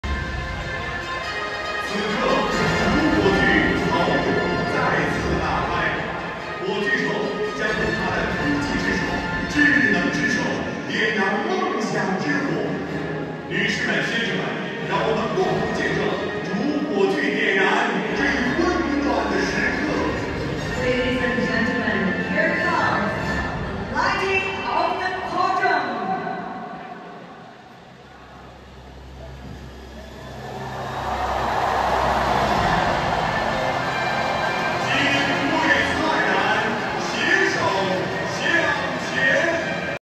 How the torchbearer, with the assistance of a robotic arm, ignites the Asian Para Games Cauldron
Here comes the lighting of the Laurels!

Let's see how the torchbearer, with the assistance of a robotic arm, ignites the Asian Para Games Cauldron!